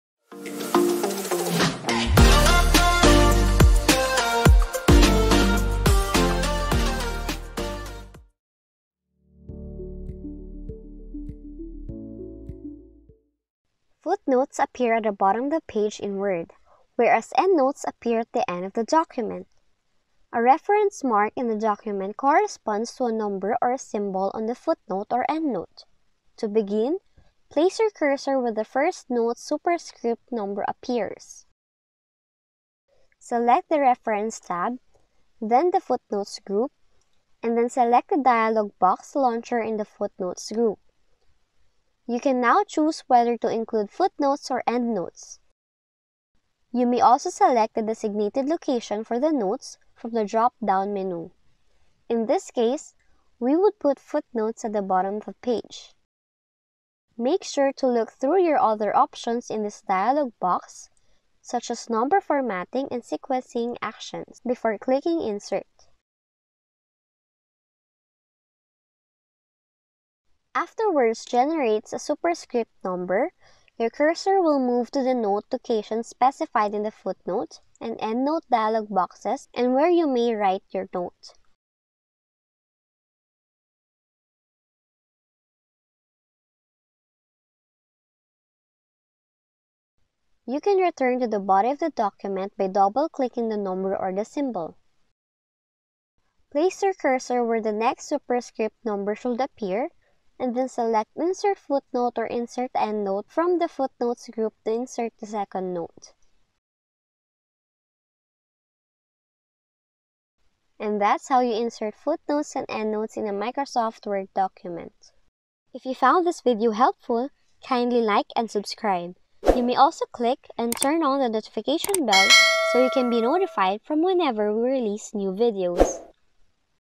[HOW TO] Insert FOOTNOTES and ENDNOTES in Microsoft WORD (Quick & Easy)
--- Footnotes appear at the bottom of the page, while endnotes appear at the end of a word. Inserting a footnote and endnote is easy!  Just follow these simple steps. ---